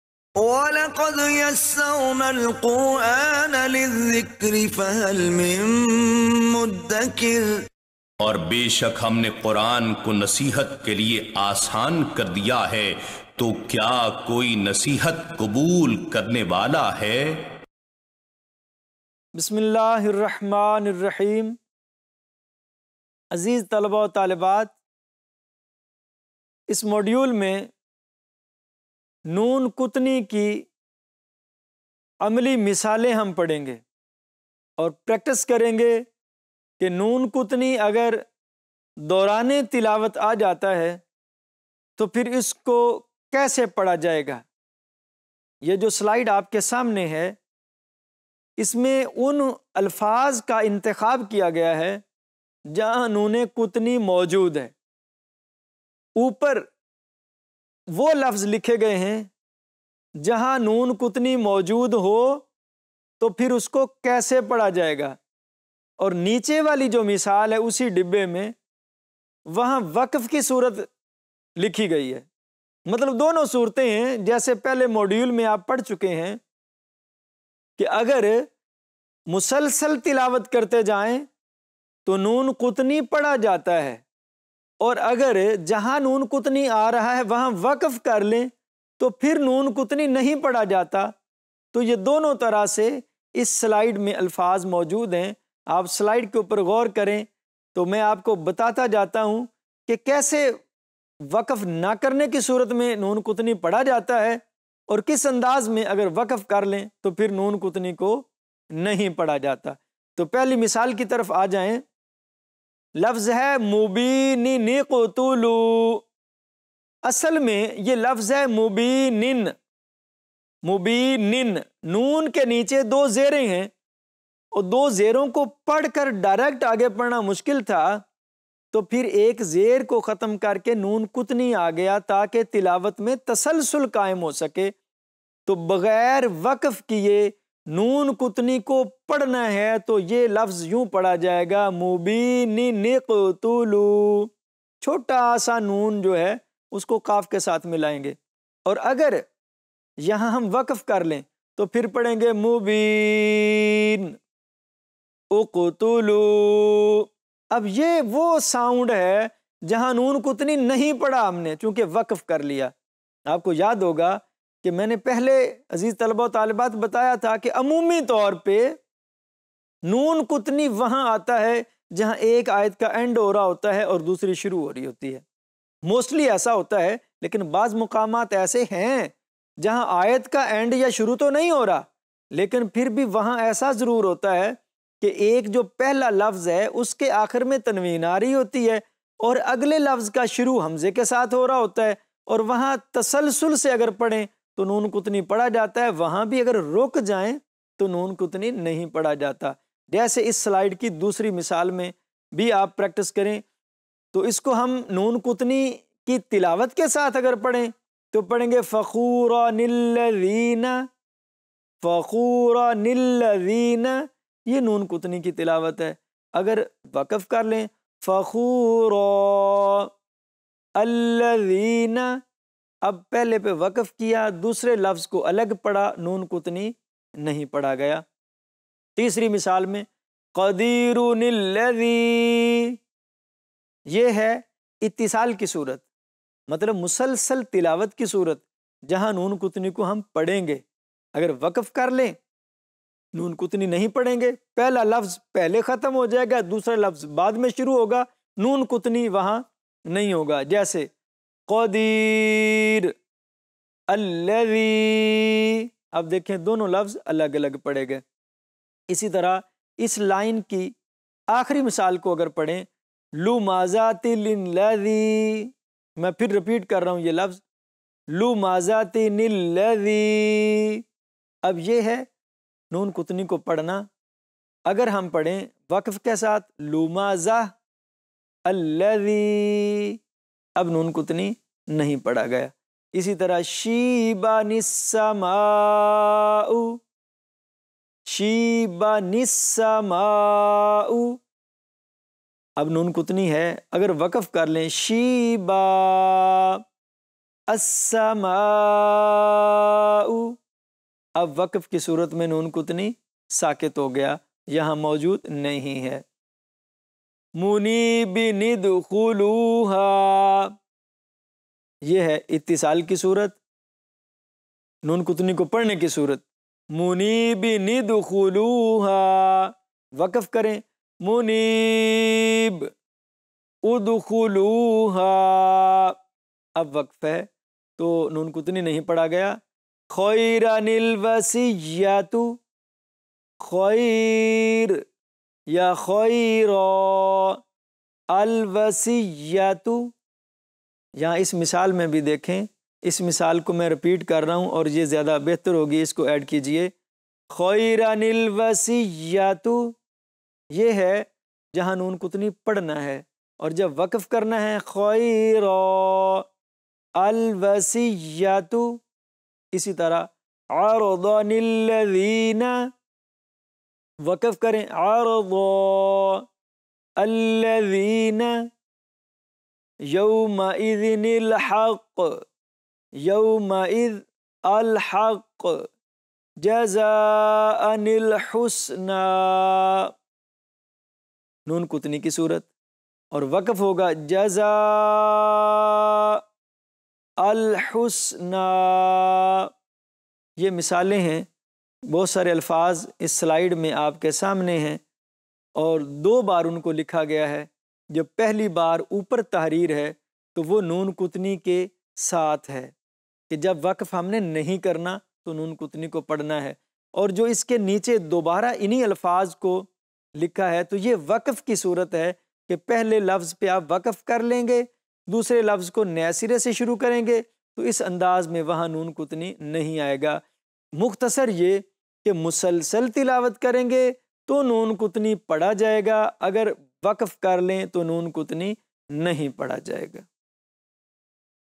"نون قطنی" کی قرآنی امثلہ | Quranic Examples of “Noon Qutni” |Arabic Quranic Text | ISL1151_Topic049
Topic049: Quranic examples of “Noon Qutni”,
"نون قطنی" کی قرآنی امثلہ
By Dr. Ali Imran